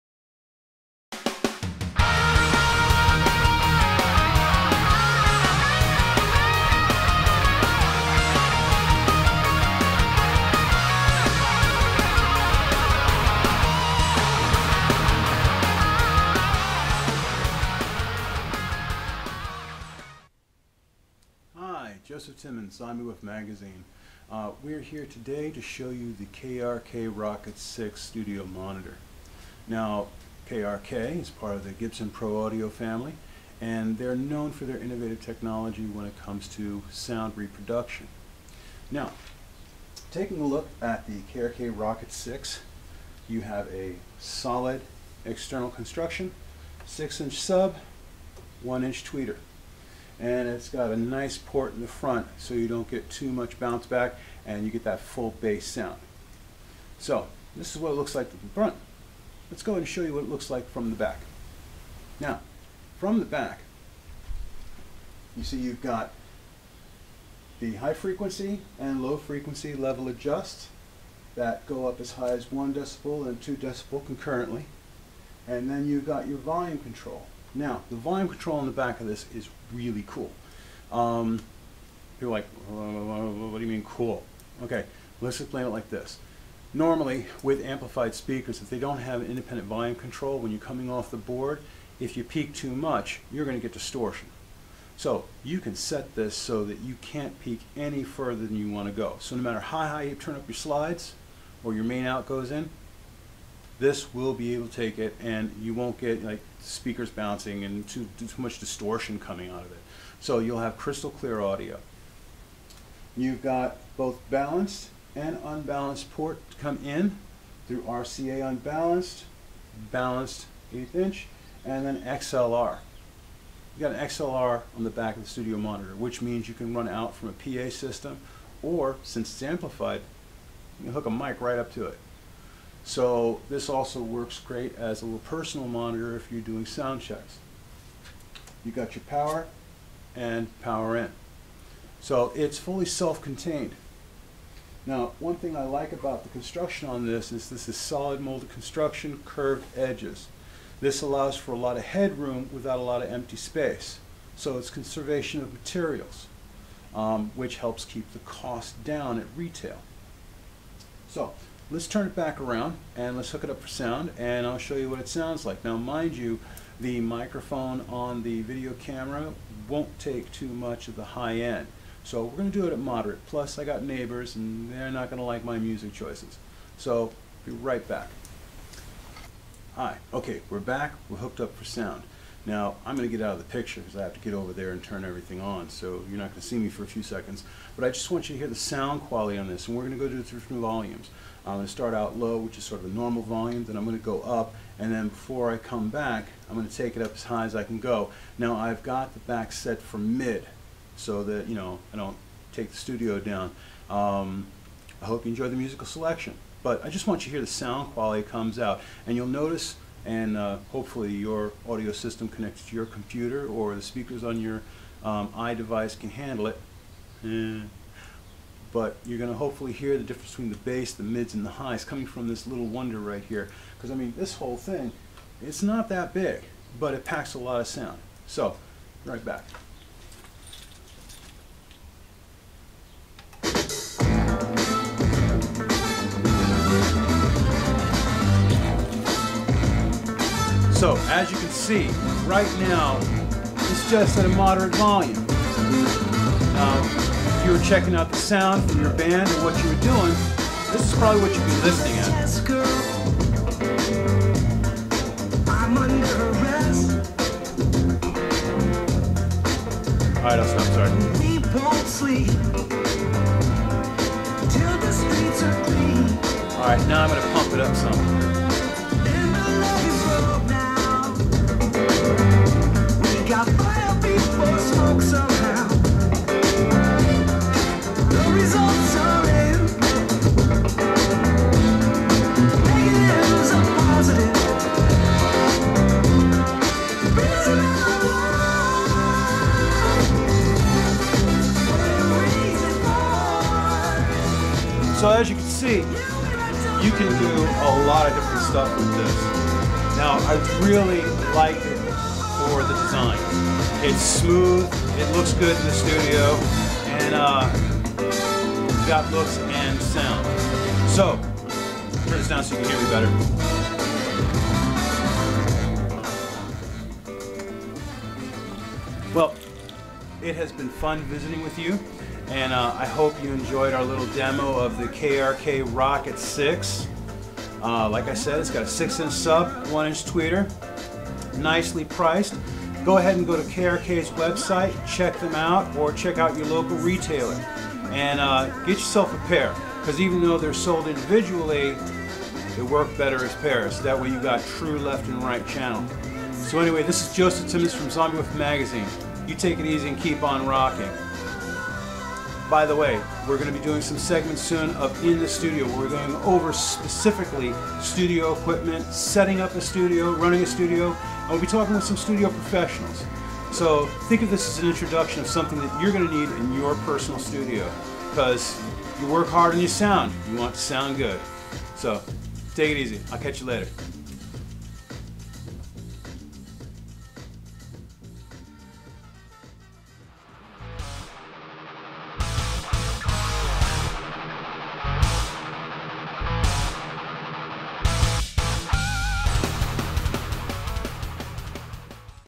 KRK ROKIT6 G3 Studio Monitors Review and Demonstration
Joseph Timmons of Xombiewoof Magazine does a demonstration of the KRK Systems KRK ROKIT6 G3 Studio Monitors, proving high quality sound is possible in small frame sound units.

for more info and retail locations.